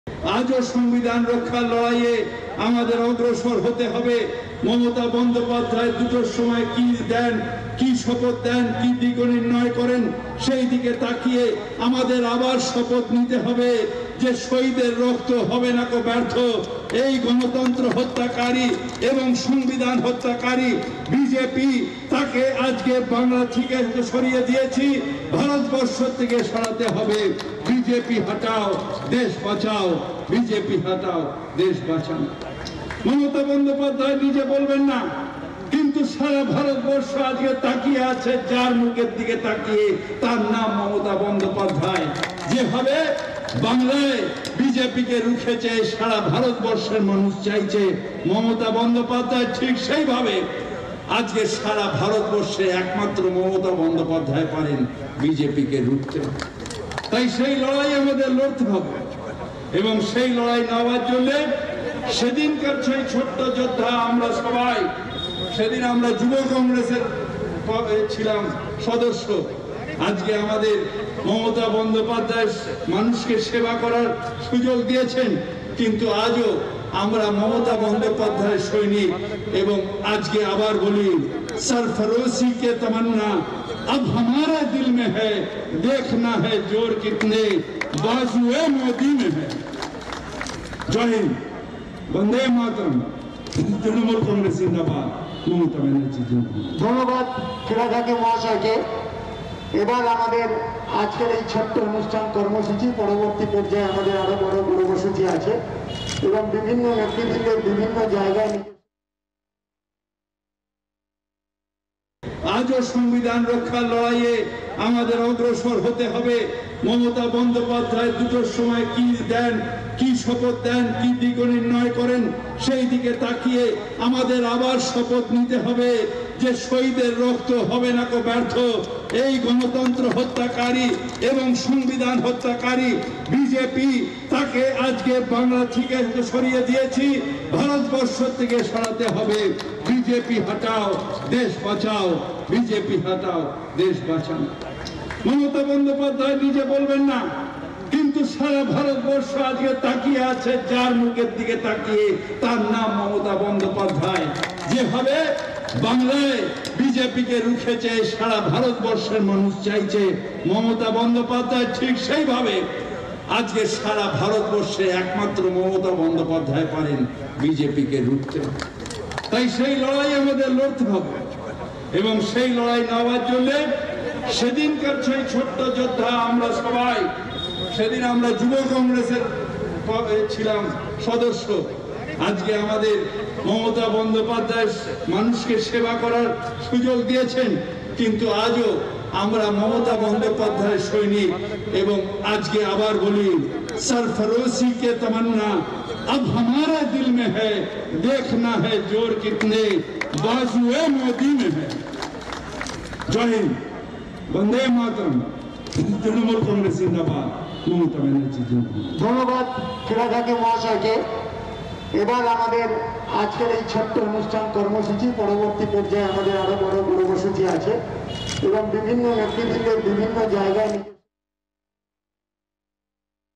'মমতা বন্দ্যোপাধ্যায় আমাদের মানুষকে সেবা করার সুযোগ দিয়েছেন। আমরা তাঁর সৈনিক।'' FirhadHakim
''মমতা বন্দ্যোপাধ্যায় আমাদের মানুষকে সেবা করার সুযোগ দিয়েছেন। আমরা তাঁর সৈনিক। মানুষের জন্য সারাজীবন কাজ করে যাবে মা-মাটি-মানুষের সরকার।''